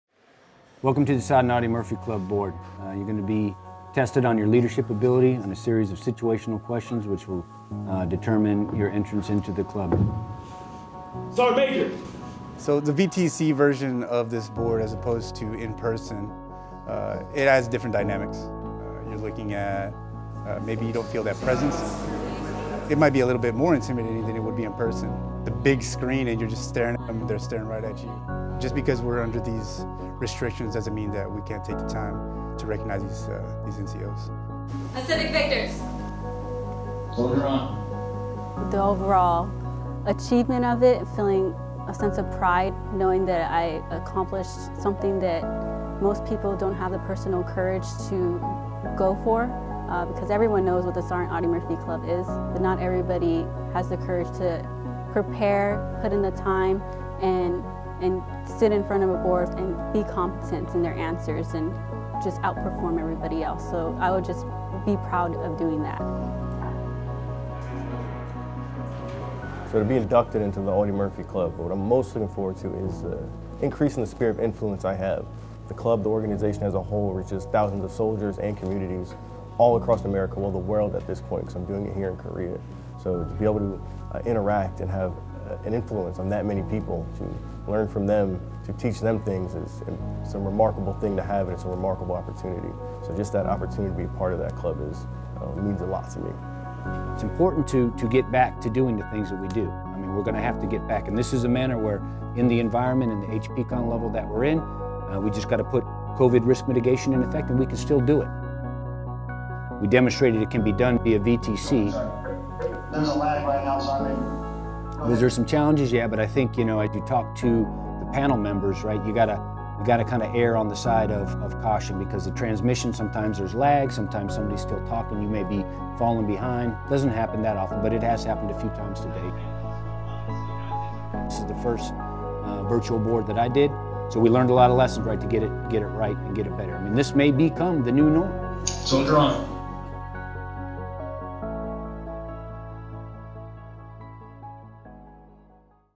Sergeant Audie Murphy Club Board
Staff Sgt. Ashley Jimenez talks about what it takes to be one of the best NCOs in the USArmy. Learn how to be a part of the Sergeant Audie Murphy Club, visit the 8th Army Pacific Victors Sergeant Audie Murphy Club - Camp Humphreys Facebook page. (Video by 20th Public Affairs Detachment)

2nd Infantry Division U.S. Forces Korea 65th Medical Brigade 19th Expeditionary Sustainment Command